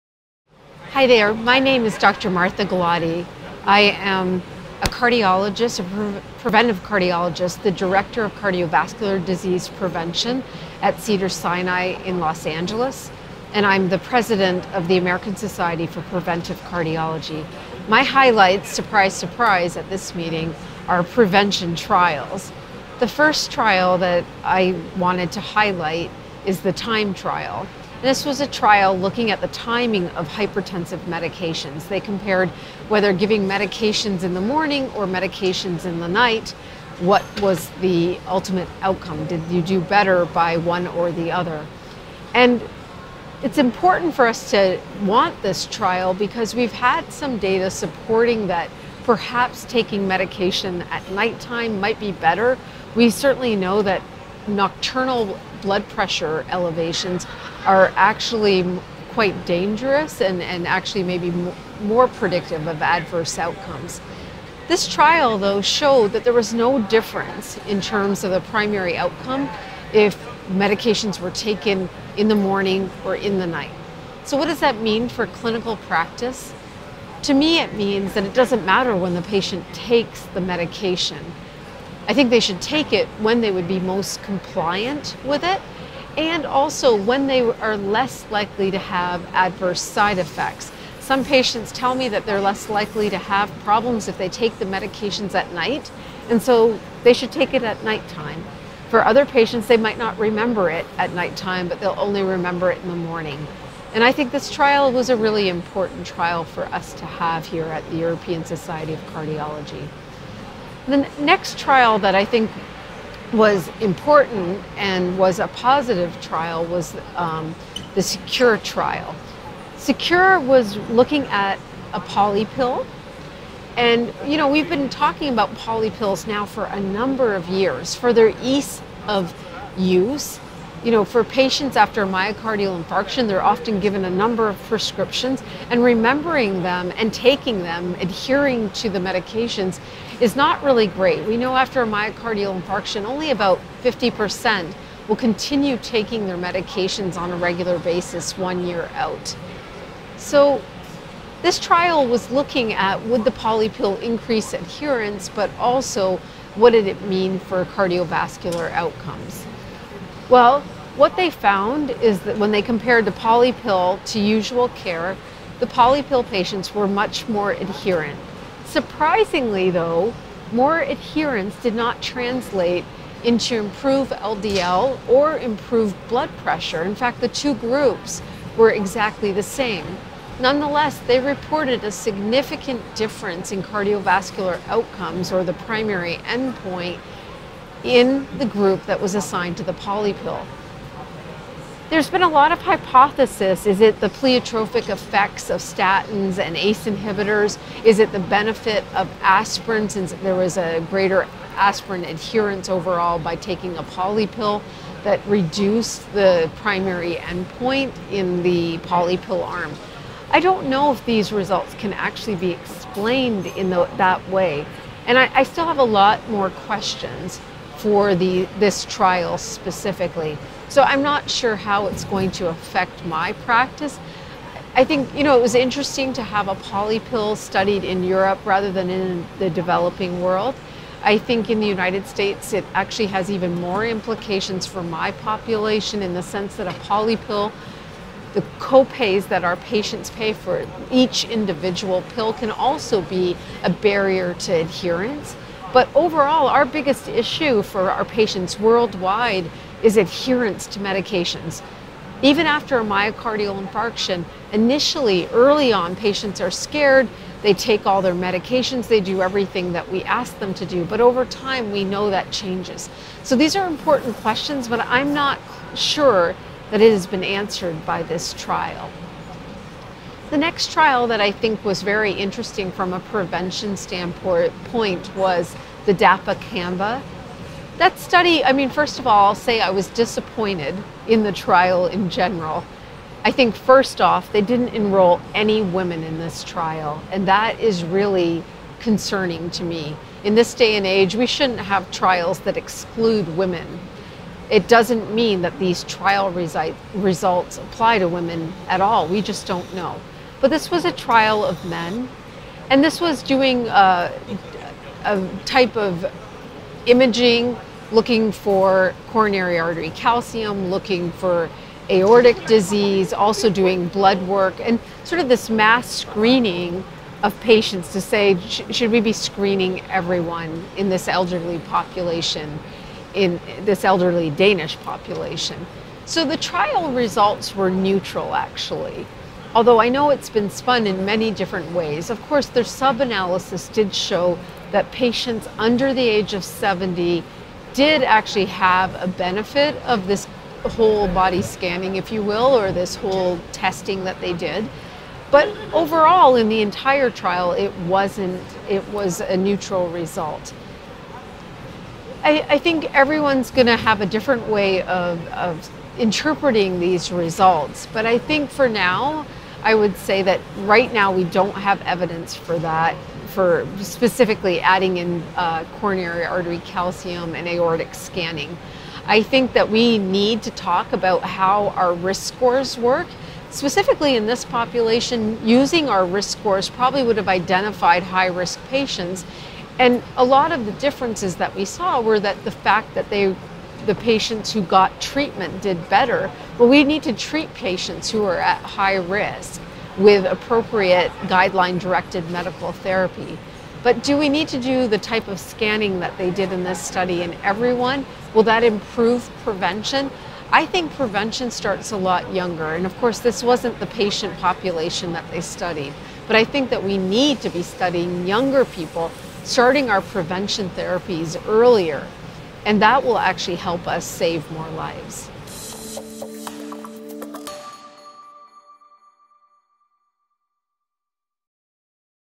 ESC 2022: 3 Trials That Will Change Your Practice With Preventive Cardiologist, Dr Martha Gulati
ESC Congress 2022 — In this short and practice-focused review, Dr Martha Gulati, Associate Director of the Barbra Streisand Women Heart Center and Director of CVD Prevention (Cedars-Sinai Medical Center, US) shares some her thoughts on the top 3 hot line trials.

Trials covered in detail include:
00:27: TIME: The Treatment in Morning versus Evening
01:57: SECURE: A polypill strategy in secondary prevention
04:31: DANCAVAS: Screening and intervention to prevent cardiovascular disease

For more ESC coverage browse our ESC Late-breaking Science Collection.

Recorded at ESC 22, Barcelona.